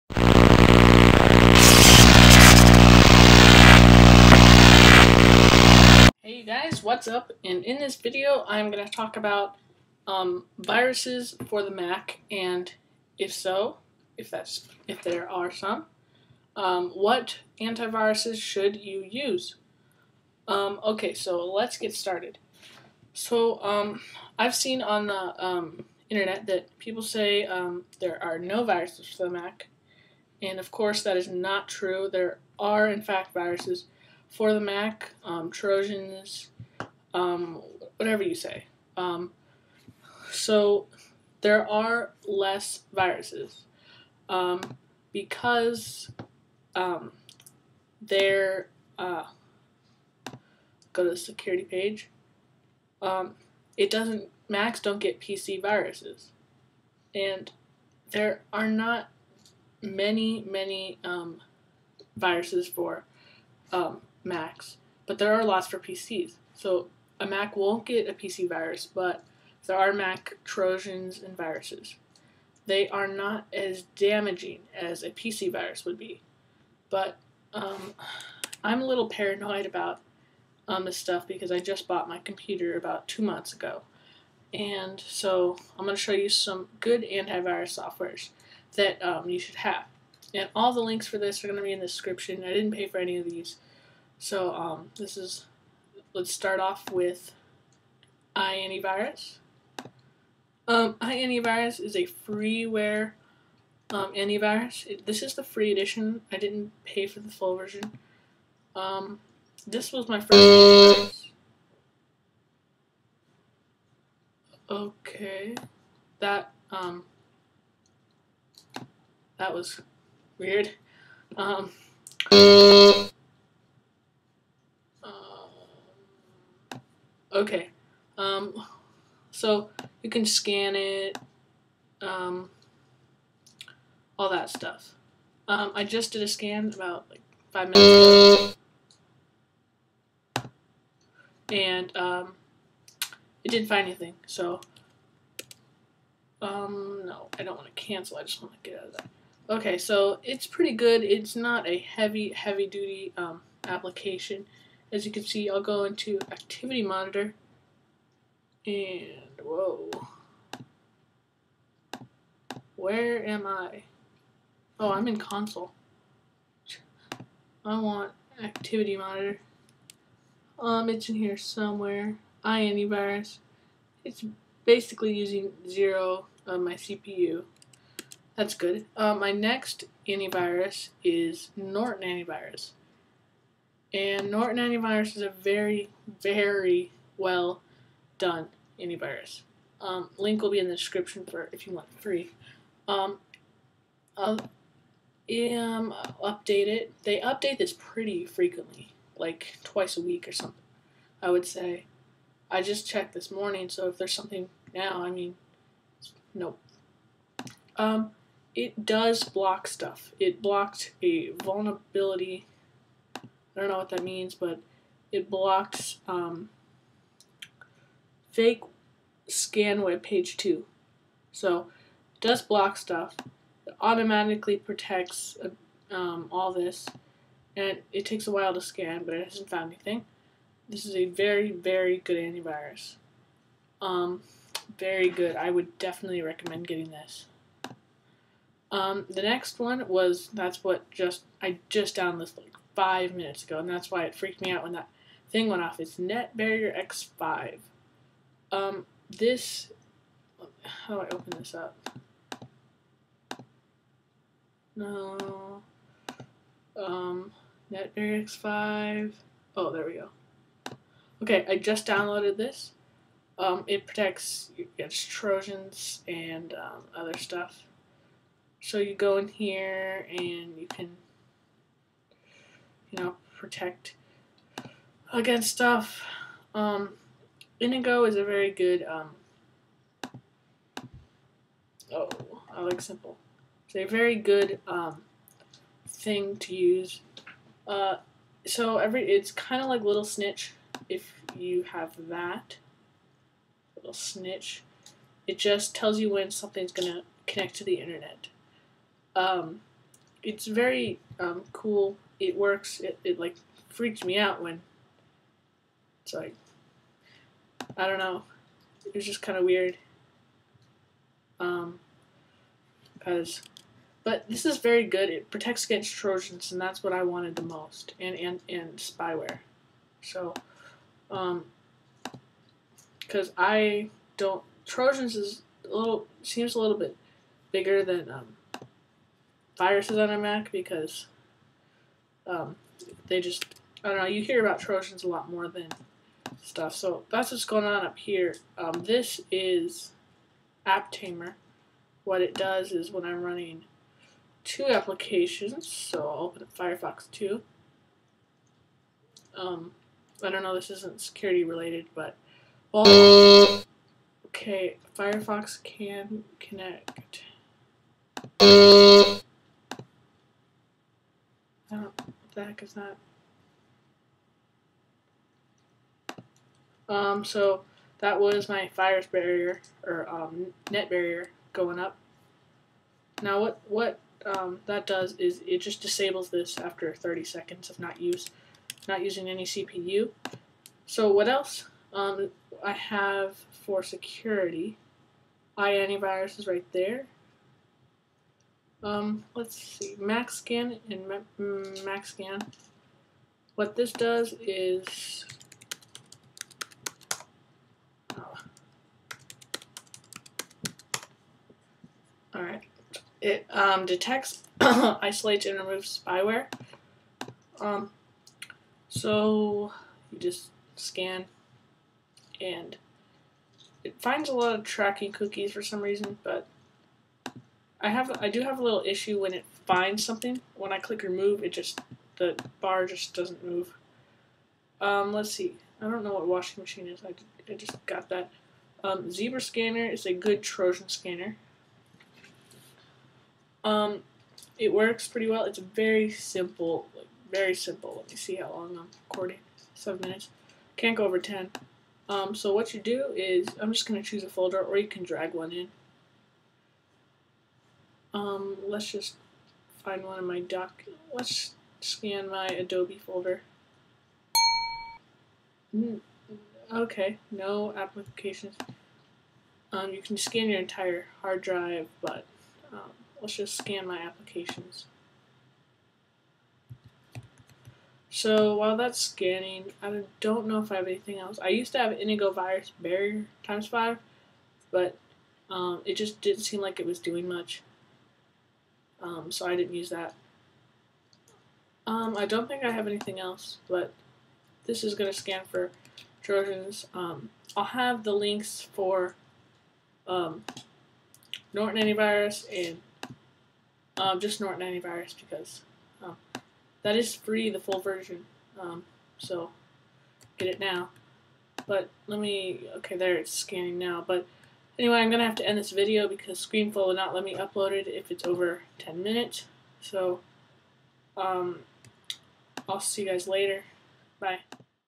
 Viruses for OS X? What Antivirus Do you Recommend? 
Mac Antivirus, Not All Of These Are Necessary, But They Are More Choices To Suit Your Preference.